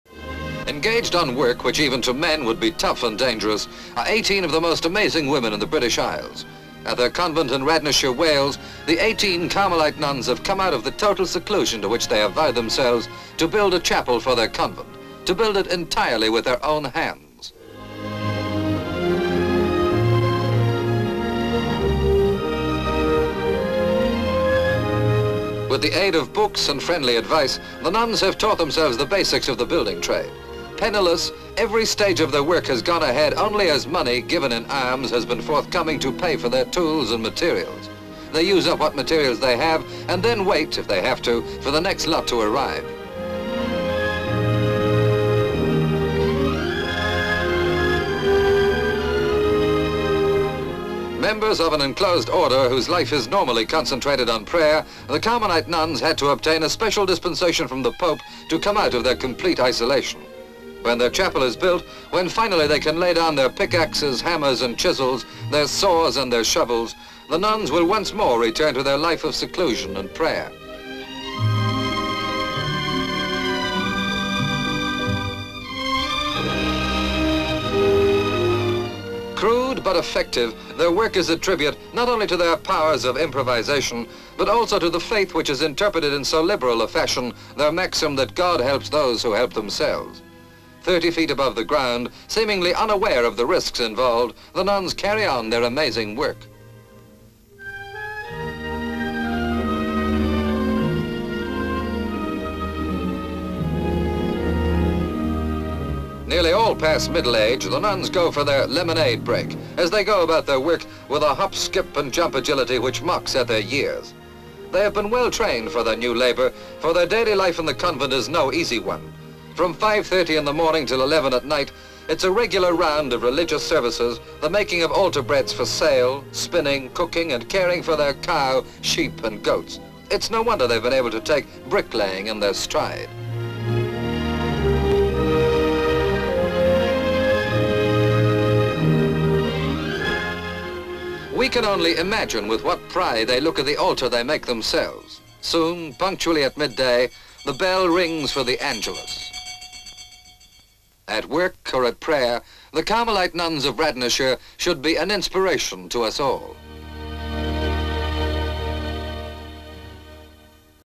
Convent Builders (1954)
Garmelite / Carmelite ? nuns build their own chapel in Radnorshire (aka Powys), Wales.

Camera following nun with watering can. She adds water to cement other nuns are mixing. C/U cement.  M/S two nuns pulling trolley with stone slabs on it.  M/S two nuns pulling trolley up to chapel building site.  M/S nuns carrying large stone slabs. Various shots nuns fixing stone slabs to walls. Various shots nuns chiselling at stone wall.  M/S nun working. C/U nun lifting wooden frame from cement which she has moulded in brick. Various shots nun cementing wall.

C/U Mother Prioress watching.  M/S head nun looking at the progress of the wall.  M/S nun measuring wall with stick.  Head nun looks on. M/S nun chipping at stone.  M/S nun cementing round by concrete steps.  C/U kitten on steps.  M/S nun cementing steps.  M/S nun walking up ladder which is by house holding drainpipe which she hands to another nun already on roof.  M/S looking down on three nuns holding ladder. Various shots nun on roof with drainpipe.

L/S nuns working on convent building.  M/S two nuns carrying lemonade.  M/S nuns putting tools down.  M/S nun hitting wall, stopping and going for a drink. High angle shot looking down on nuns taking their refreshment. C/U nun drinking lemonade.  L/S nuns working on enclosure wall.  M/S head nun.  Various shots of nuns fitting iron window frame into wall.  More shots nuns building wall.  M/S head nun ringing bell. Various shots nuns stopping work and praying. Camera pans down to rosary beads head nun is holding in hand.

Note: newspaper articles about the nuns on file.
 FILM ID:1335.09